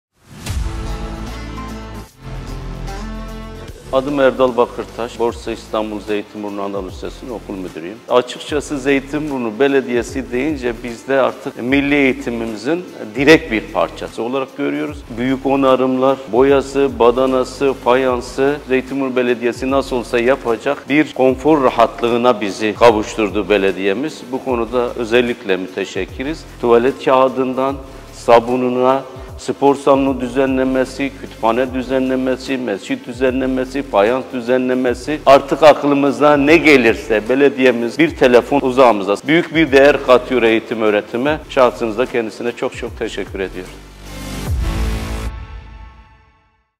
Okullarımızın Onarımı, Bahçe ve Spor Alanlarının Düzenlenmesi
“Zeytinburnu Belediyesi’ni Millî Eğitim Bakanlığı’nın bir parçası olarak görüyoruz.”
Okullarımız, evlatlarımız için yaptıklarımızı özetleyen bir cümle.
Şahitlerinden biri, Erdal Bakırtaş anlatıyor.